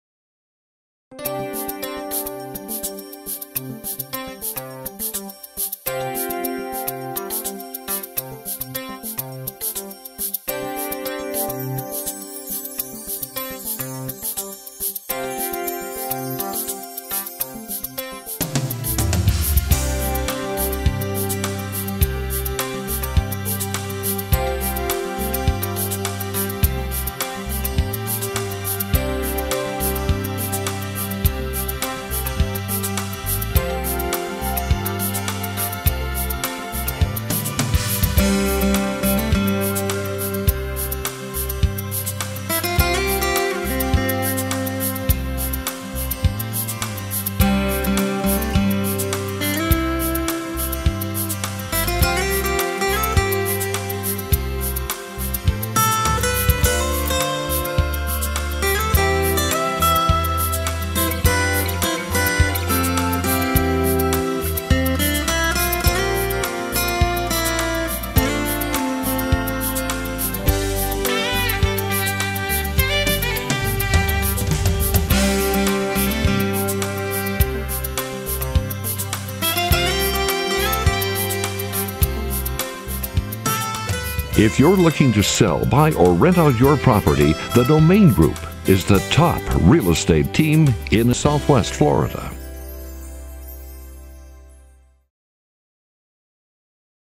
Cascades Video Tour Estero, FL
Cascades in sunny Estero, Florida is an active 55 or over community with plenty of activities for retirees.

Learn More About the Community and See All of the Homes Currently Listed for Sale in the Cascades